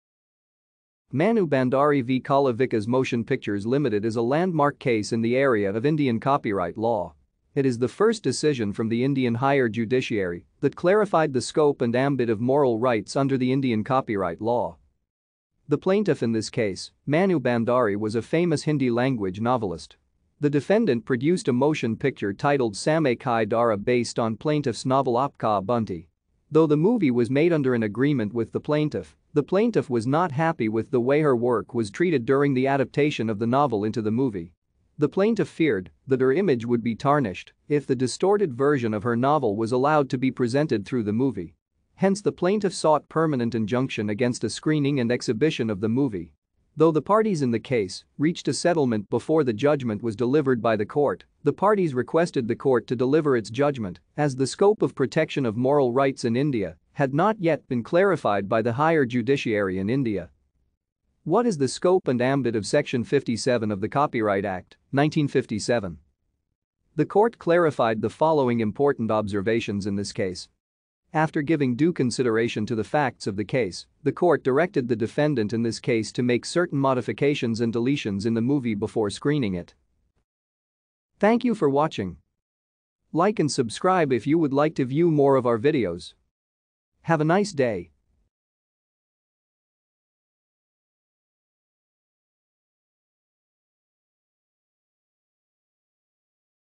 Manu Bhandari V. Kala Vikas Motion Pictures Ltd
Mannu Bhandari v. Kala Vikas Motion Pictures Ltd  is a landmark case in the area of Indian copyright law. It is the first decision from the Indian higher judiciary that clarified the scope and ambit of moral rights under the Indian copyright law.